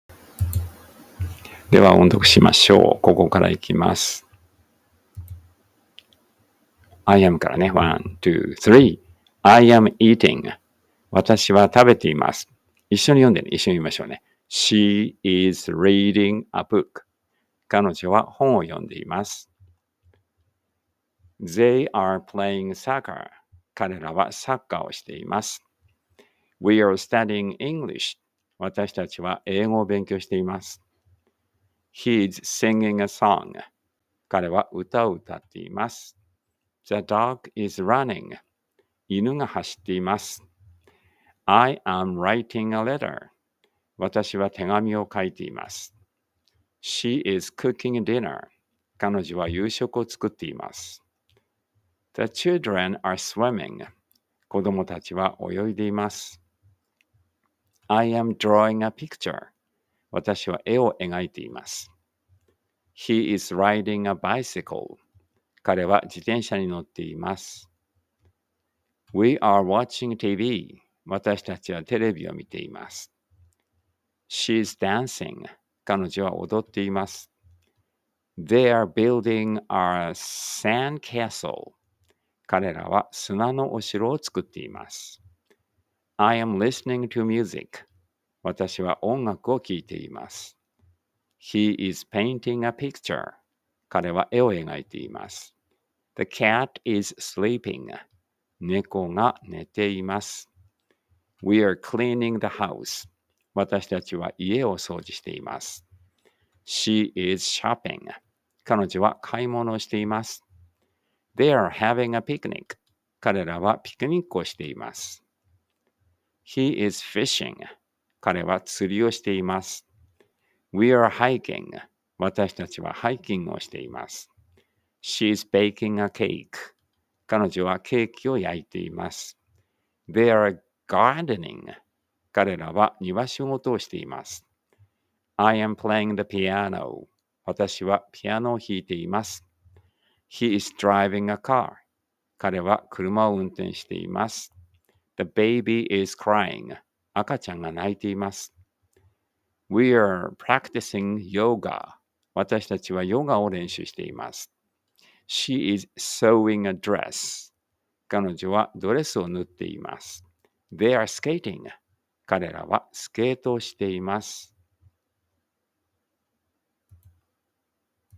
えいご おんどく 270
1. **I am eating.**
   - 私は食べています。

2. **She is reading a book.**
   - 彼女は本を読んでいます。

3. **They are playing soccer.**
   - 彼らはサッカーをしています。

4. **We are studying English.**
   - 私たちは英語を勉強しています。

5. **He is singing a song.**
   - 彼は歌を歌っています。

6. **The dog is running.**
   - 犬が走っています。

7. **I am writing a letter.**
   - 私は手紙を書いています。

8. **She is cooking dinner.**
   - 彼女は夕食を作っています。

9. **The children are swimming.**
   - 子供たちは泳いでいます。

10. **I am drawing a picture.**
    - 私は絵を描いています。

11. **He is riding a bicycle.**
    - 彼は自転車に乗っています。

12. **We are watching TV.**
    - 私たちはテレビを見ています。

13. **She is dancing.**
    - 彼女は踊っています。

14. **They are building a sandcastle.**
    - 彼らは砂のお城を作っています。

15. **I am listening to music.**
    - 私は音楽を聴いています。

16. **He is painting a picture.**
    - 彼は絵を描いています。

17. **The cat is sleeping.**
    - 猫が寝ています。

18. **We are cleaning the house.**
    - 私たちは家を掃除しています。

19. **She is shopping.**
    - 彼女は買い物をしています。

20. **They are having a picnic.**
    - 彼らはピクニックをしています。

21. **He is fishing.**
    - 彼は釣りをしています。

22. **We are hiking.**
    - 私たちはハイキングをしています。

23. **She is baking a cake.**
    - 彼女はケーキを焼いています。

24. **They are gardening.**
    - 彼らは庭仕事をしています。

25. **I am playing the piano.**
    - 私はピアノを弾いています。

26. **He is driving a car.**
    - 彼は車を運転しています。

27. **The baby is crying.**
    - 赤ちゃんが泣いています。

28. **We are practicing yoga.**
    - 私たちはヨガを練習しています。

29. **She is sewing a dress.**
    - 彼女はドレスを縫っています。

30. **They are skating.**
    - 彼らはスケートをしています。